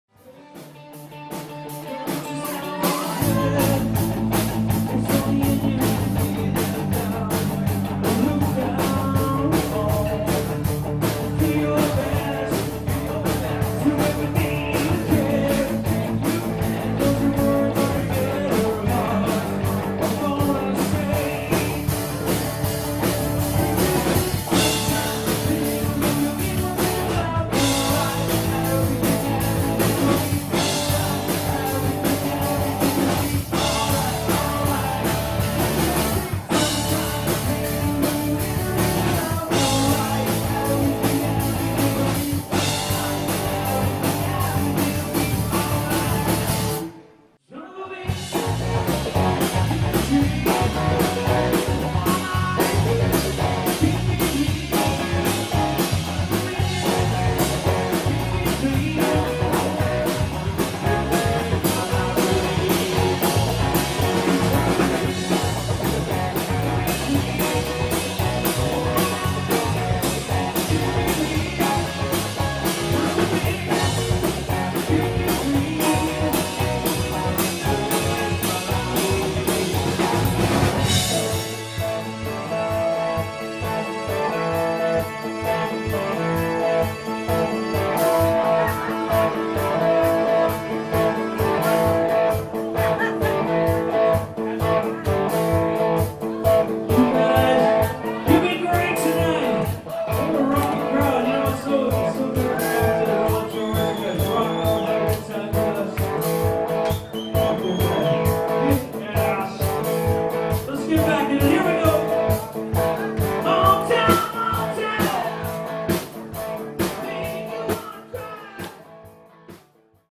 ajmdrums - Soul Seed
Short montage from Soul Seed's show at The Royal George Hotel (Winnipeg MB), June 27th 2019.